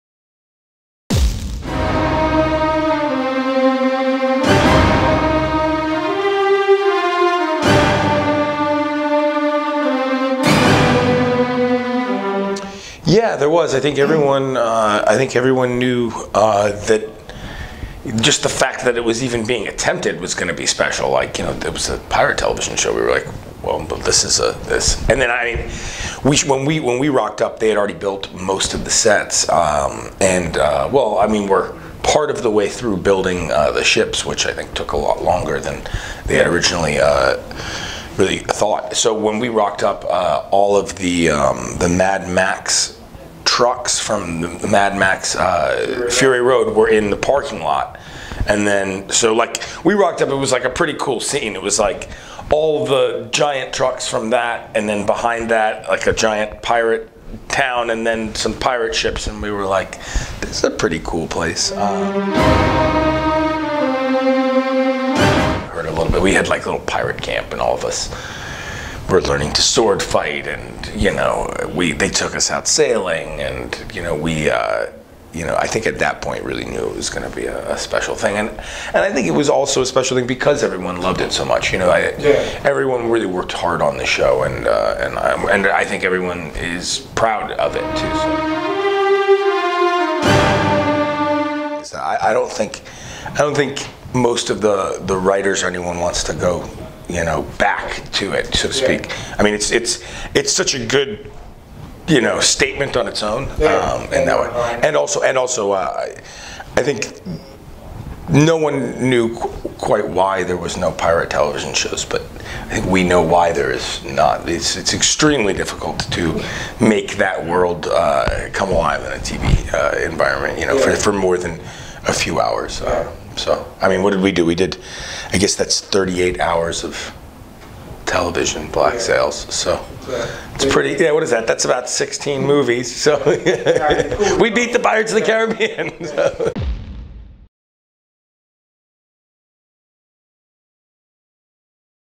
Zach McGowan talks Black Sails
We caught up with Black Sails pirate Zach McGowan at Oz Comic-Con, Melbourne to talk about his time on the swashbuckling series as Captain Charles Vane.

STACK is JB Hi-Fi's essential monthly guide to cinema, games, music and DVDs.